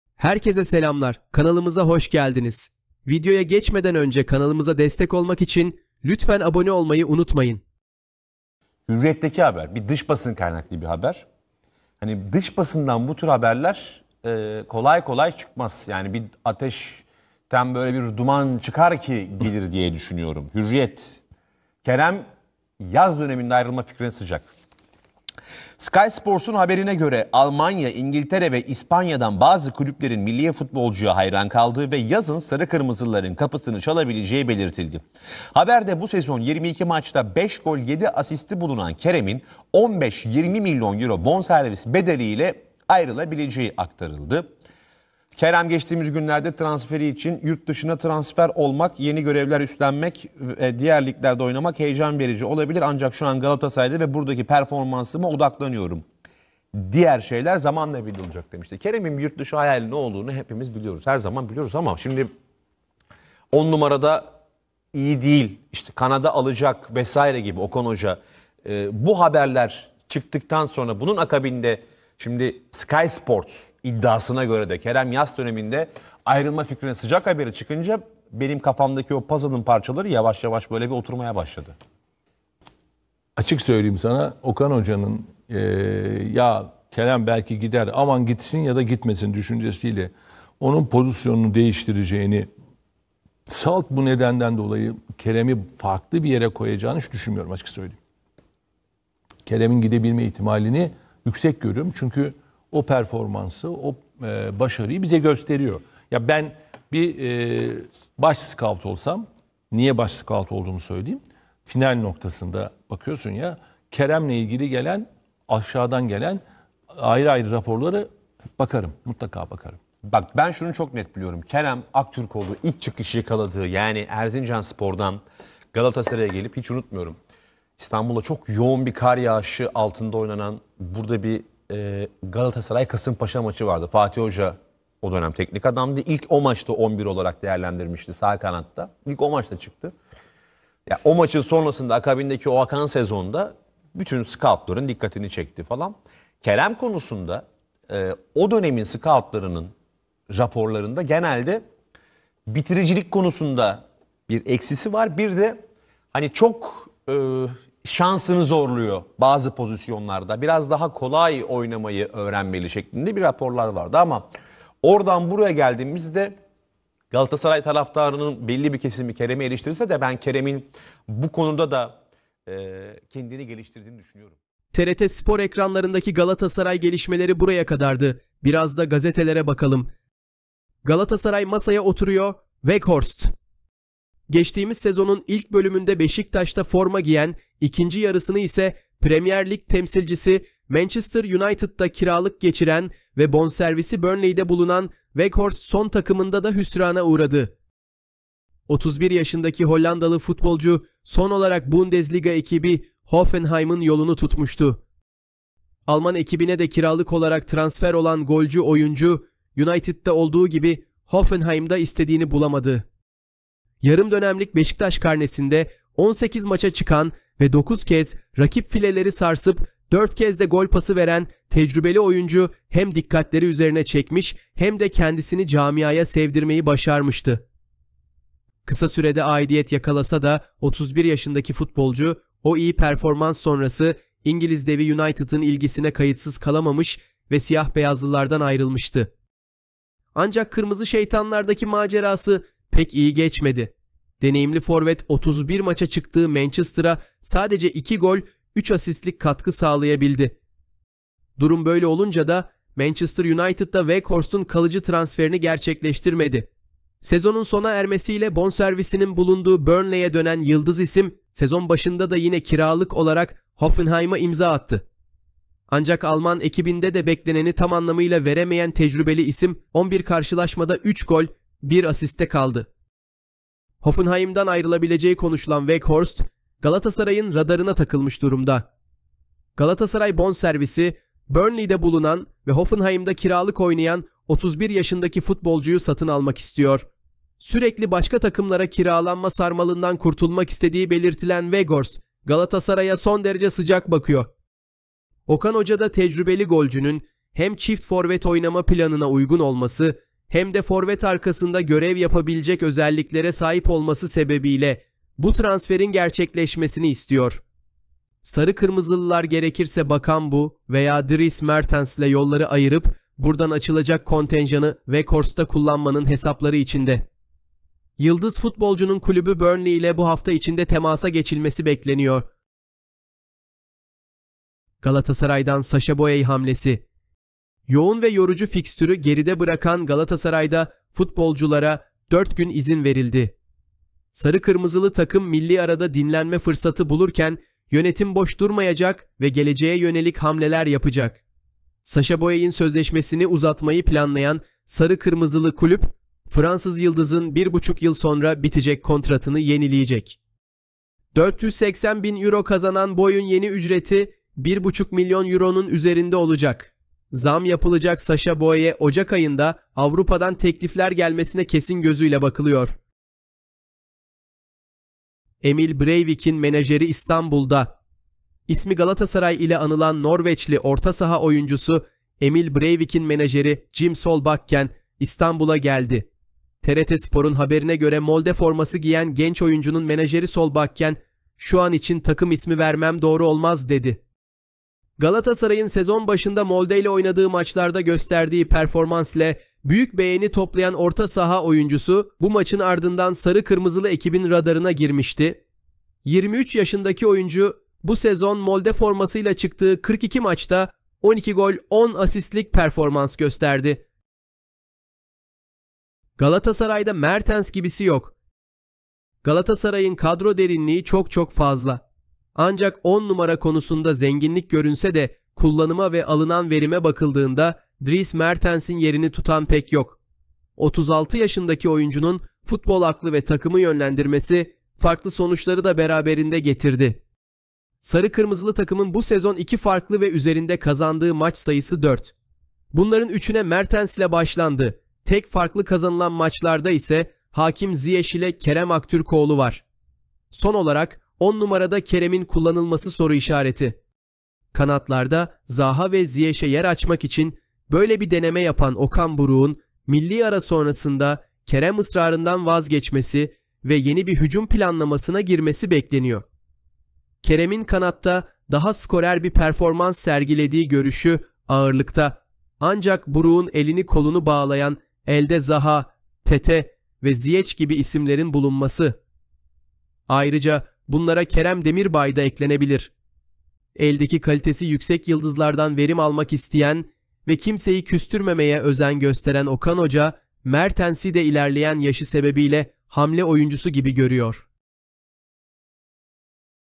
Canlı Yayında Belli Oldu !! Menajeri İstanbul'a Geldi !! l Weghorst & Emil Breivik l GALATASARAY
Galatasaray Ocak ayında yine büyük bir transfer dönemine imza atmak istiyor ve gündemde Weghorst ve Emil Breivik var.
İyi seyirler...